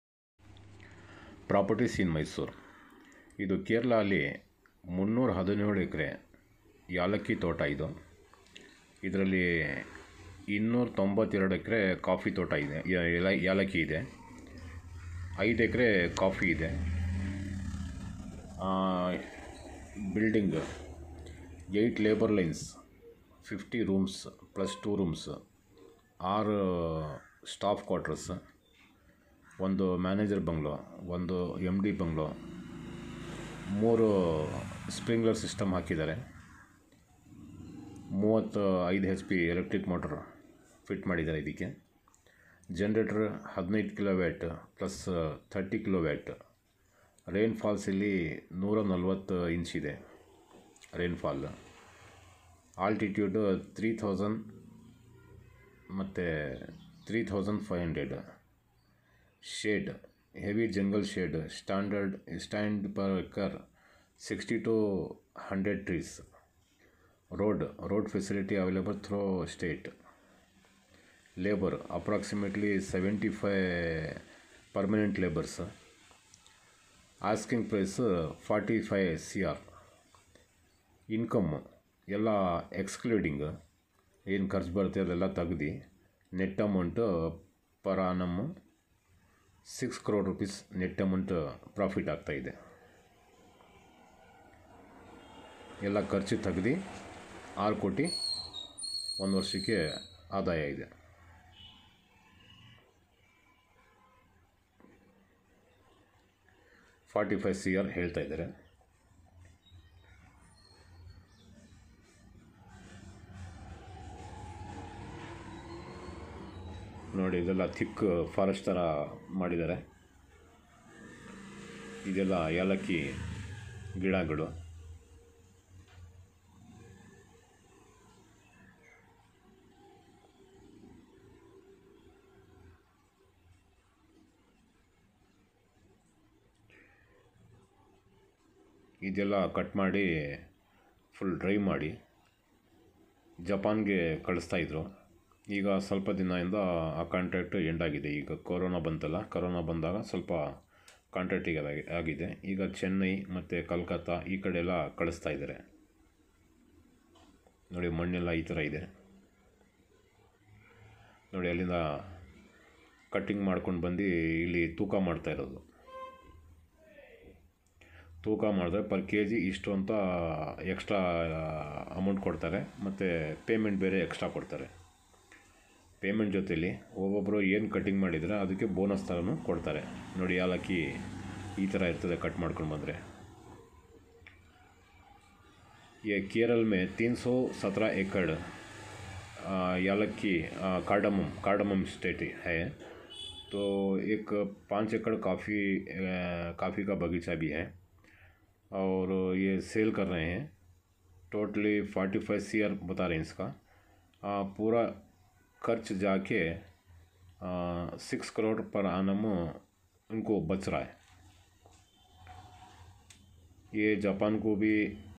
December 12, 2021
317 Acres Estate for sale at Kerala . It is growing CARDAMOM 292 ACRES , COFFEE 5 ACRES , GRASSLA .
ND 20 ACRES .

( Buildings )  8 Labour lines - 50 Rooms + 2 Rooms , 6 Staff Quarters + 1 .
Large Smoke House ( 3 floors ) + 3 Dryers [ Kardi Dryer 500 Kg , 750 Kg Dryer , 1000 Kg ]

( Quarters )  1 Manager's Bungalow , 1 MD Bungalow .

( Sprinkler )  3 Systems, Covering 150 Acres , 1 35 HP Electric Motor 3 Phase with Agri Connection .

( Generator )  16 Kw + 30 Kw .

( Rain Fall )  Average 140"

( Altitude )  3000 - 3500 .

( Shade )    Heavy Jungle Shade . Stand per Acre 60 - 100 Trees .

( Road )   Road Facility Available through Estate .

( Labor )  Approximately  75 permanent Labours .

Asking price Rs - 45,00,00,000/-

Income per Anum Rs - 6,00,00,000/- net amount Excluding labour expensive and all .